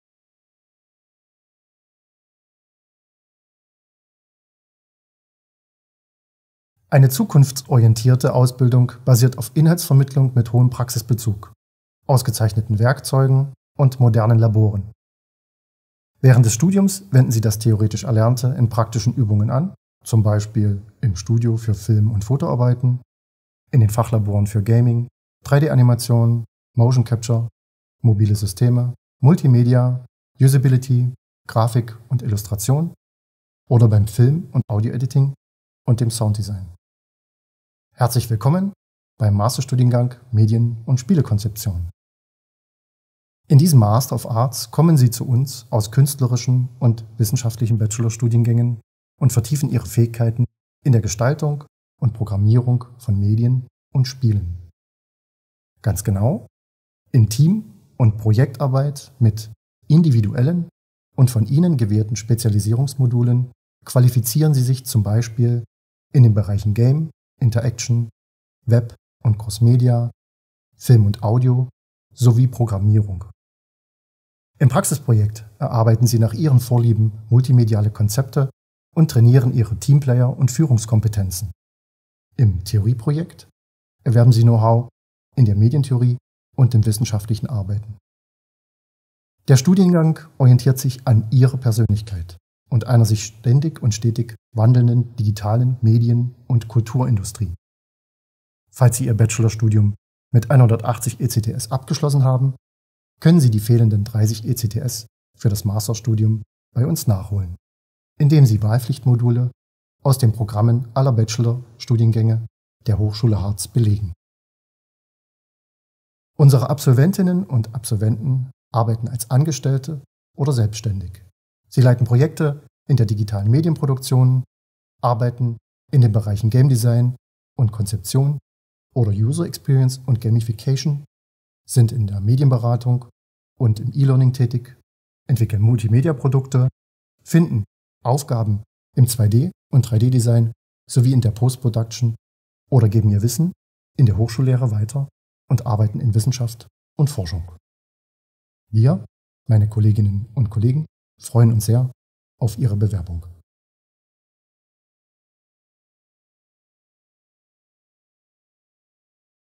Medien- und Spielekonzeption an der Hochschule Harz studieren
Im Master-Studiengang Medien- und Spielekonzeption stehen die Ideen und Konzepte unserer Studierenden im Mittelpunkt. Es werden Kenntnisse in Team- und Projektarbeit mit individuellen Spezialisierungsmodulen vertieft – dies qualifiziert die Studierenden für die Konzeption von Medienprodukten. Im Fach Programmier- und Gestaltungskonzepte werden die Qualifikationen im Bereich der Programmierung sowie in der Gestaltung von Medien und Spielen erweitert. Im Praxisprojekt werden nach den Vorlieben der Studierenden multimediale Konzepte erarbeitet.

Mehr Informationen zum Master-Studium Medien- und Spielekonzeption an der Hochschule Harz